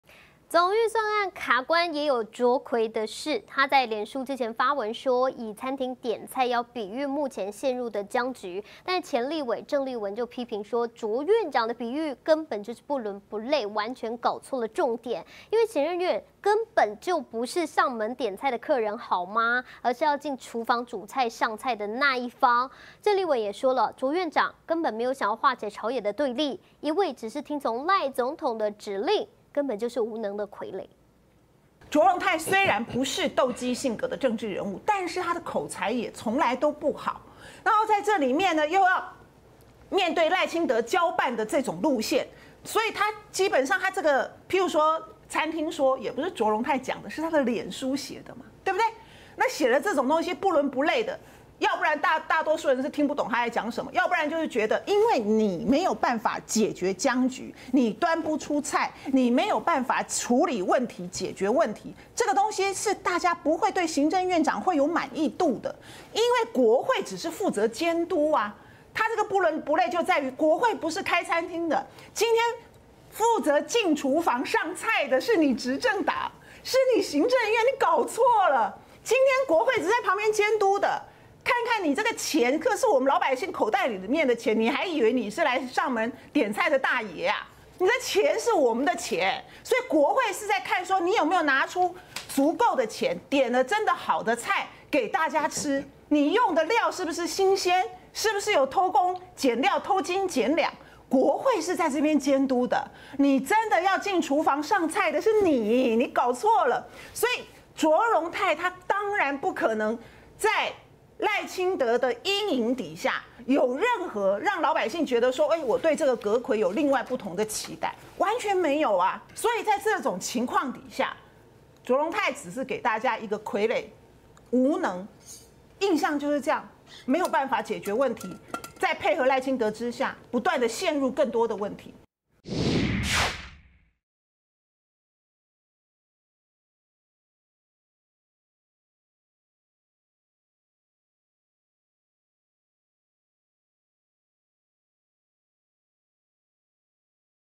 【每日必看】卓榮泰"點菜說"不倫不類 鄭麗文:就是無能傀儡 20241022
中央政府總預算案審查持續卡關，行政院長卓榮泰在臉書發文，
以「餐廳點菜」來比喻目前的僵局。

前立法委員鄭麗文批評，卓院長的比喻根本就是不倫不類、完全搞錯重點，
行政院並不是上門點菜的客人，而是該要進廚房煮菜、上菜的那一方；
鄭麗文認為，卓院長並沒有想要化解朝野對立，
根本只是一味聽從賴總統指令的無能傀儡。